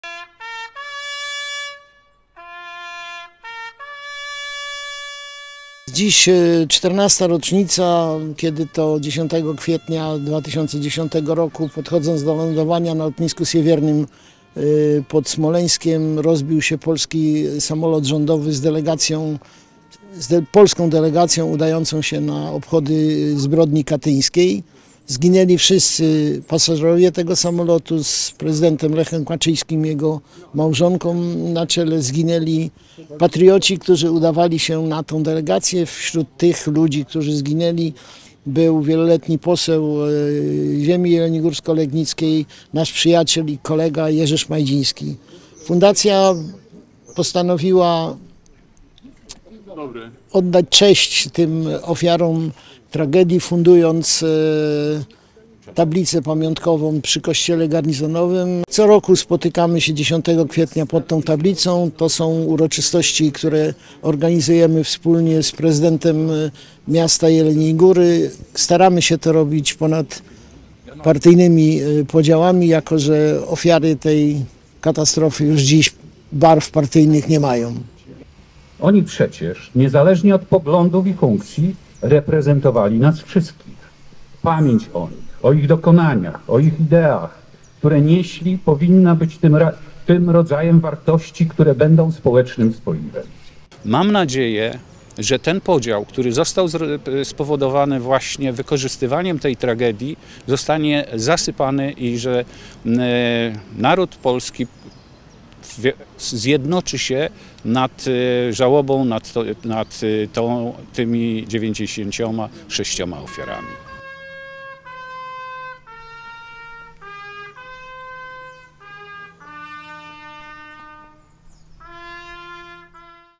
Obchody 14. rocznicy katastrofy Smoleńskiej.
Dziś (10.04.) odbyła się krótka uroczystość obchodów katastrofy Smoleńskiej. Oto krótka relacja z tego wydarzania.